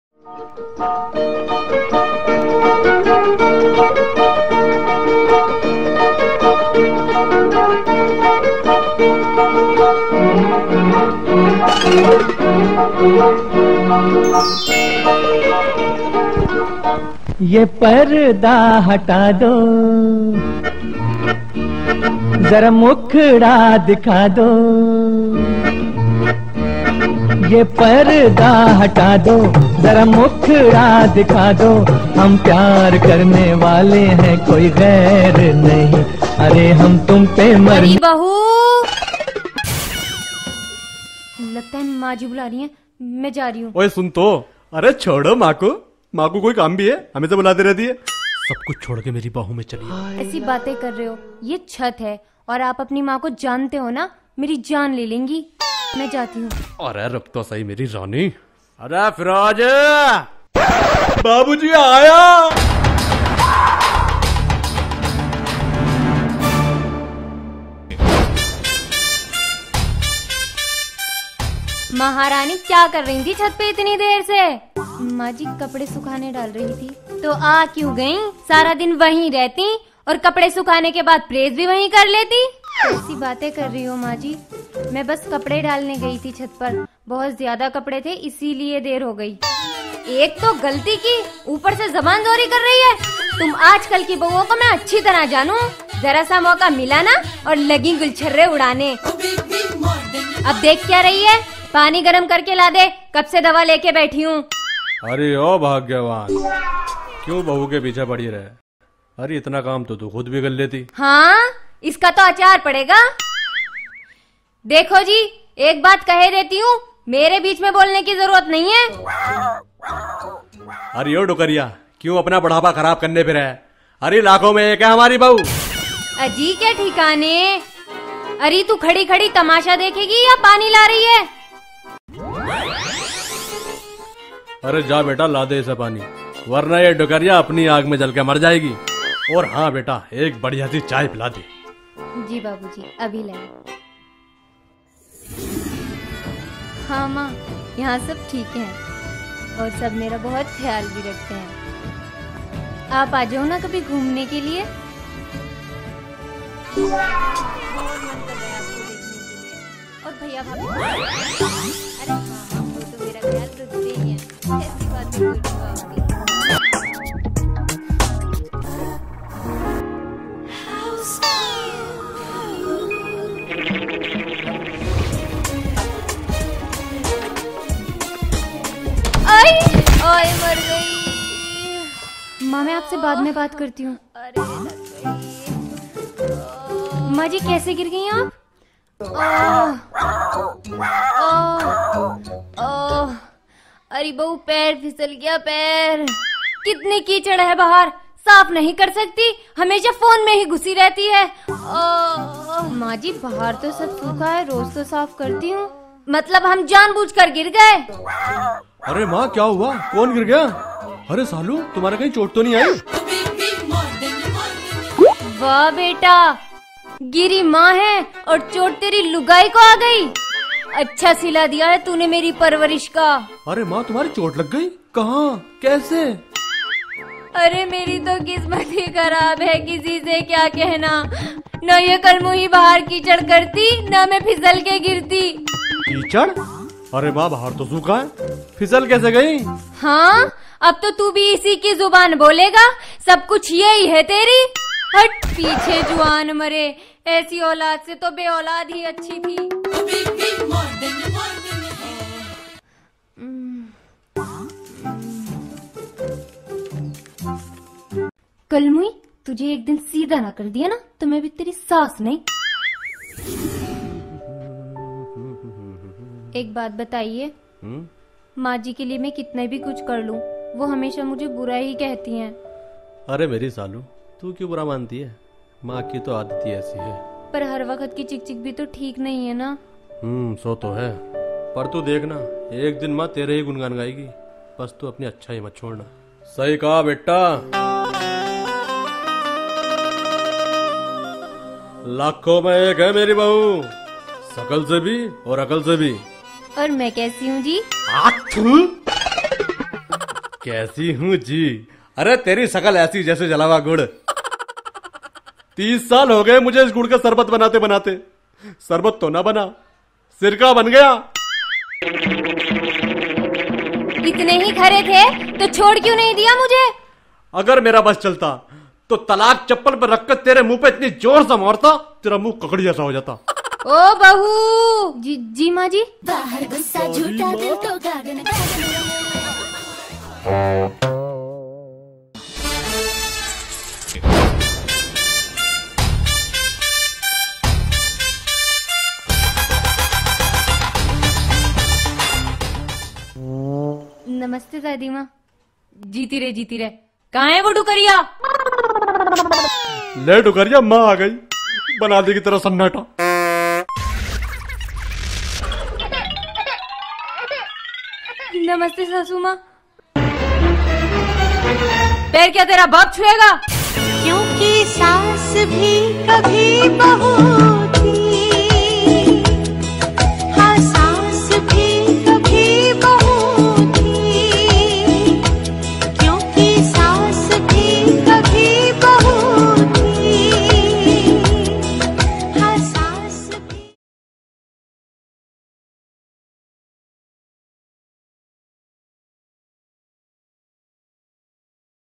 Desi Saas/Modern Bahu | Full Entertainment | Firoj Chaudhary | Comedy 2018
Dosto ye Comedy video Family Friendly hai | jisme Saas Bahu ka majedaar Drama dikhaya gaya hai. sabhi matayen or behnne jaroor dekhen. or share bhi karen.